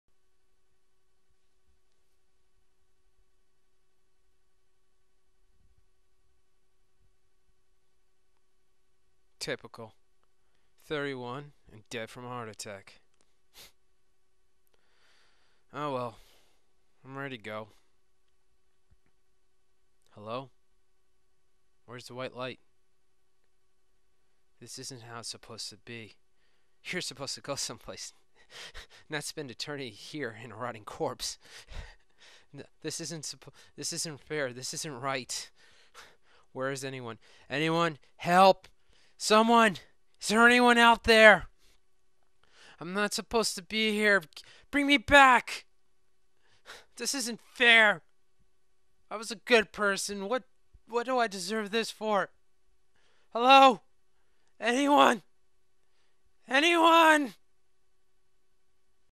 Stuck
I entered this into AMC's annual Halloween film contest of making a 1:00 minute film or less. This year was one's greatest fear.